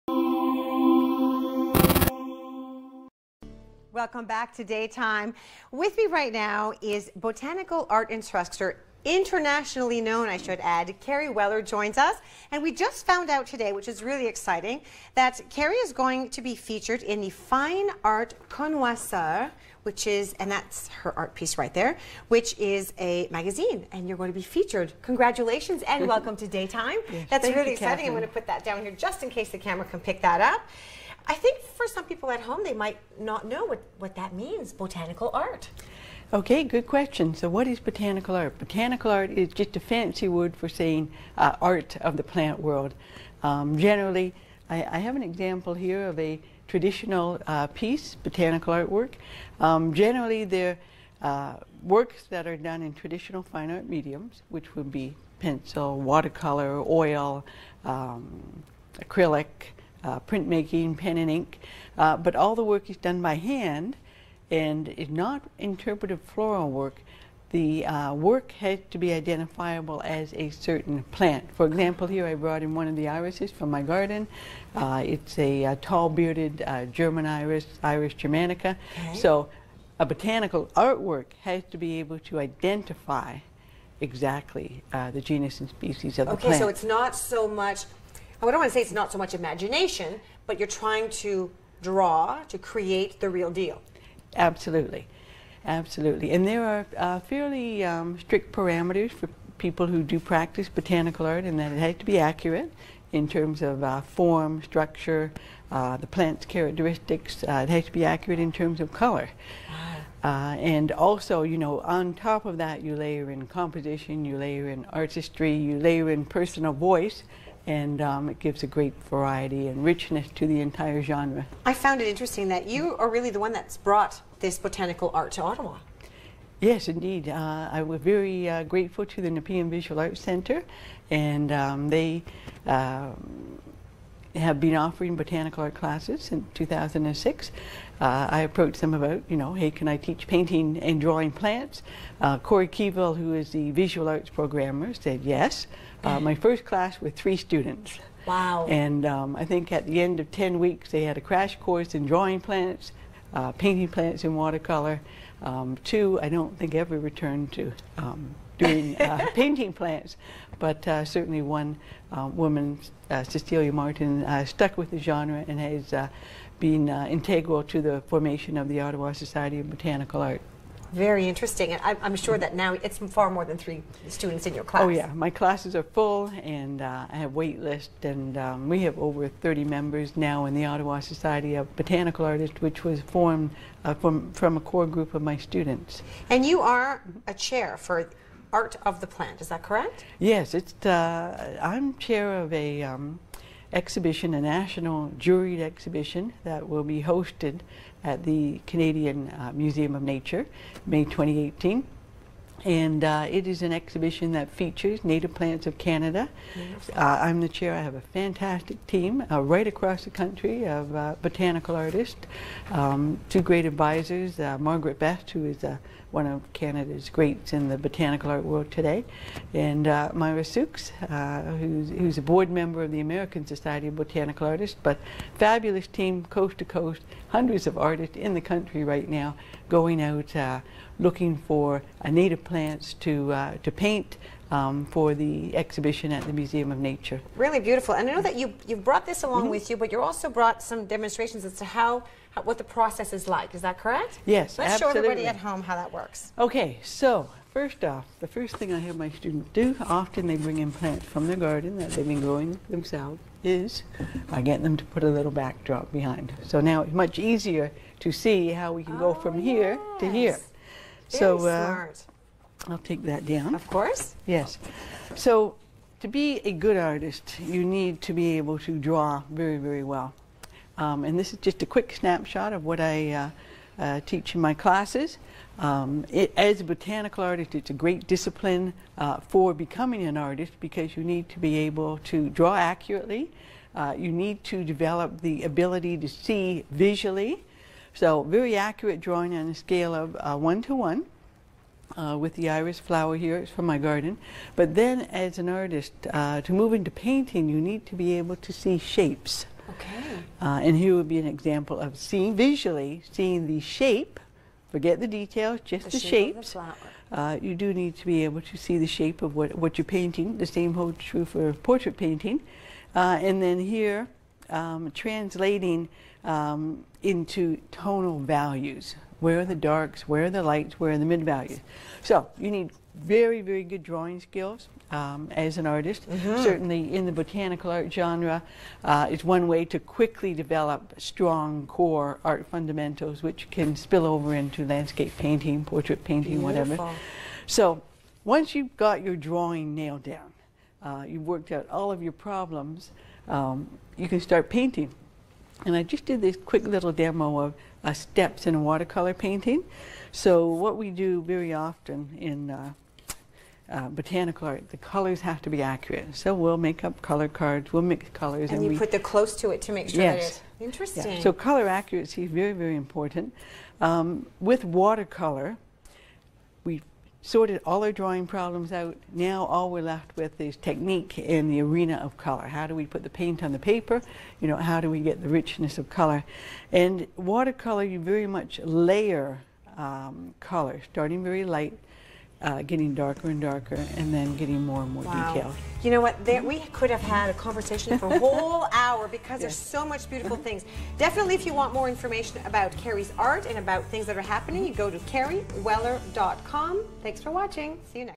Learning about Botanical Art
Artist Kerri Weller talks about Botanical Art and the classes she offers at the Nepean Visual Arts Centre.